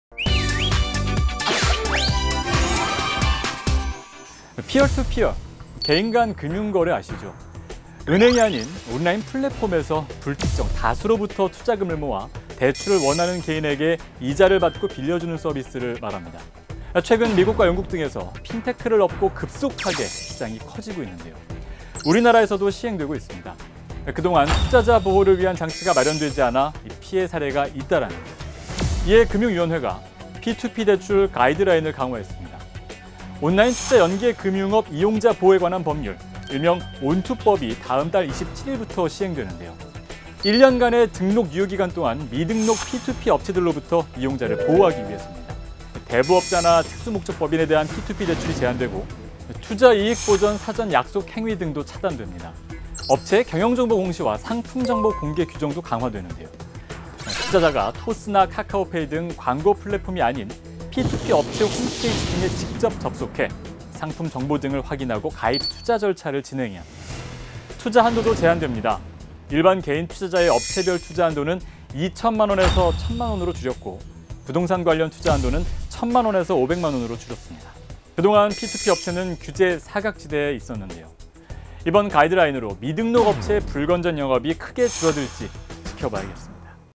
개인간 금융거래 아시죠? [S&NEWS]
개인간 금융거래 아시죠?
은행이 아닌 온라인 플랫폼에서
불특정 다수로부터 투자금을 모아

대출을 원하는 개인에게
이자를 받고 빌려주는
서비스를 말합니다.

최근 미국과 영국 등에서
핀테크를 업고 급속하게
시장이 커지고  있습니다.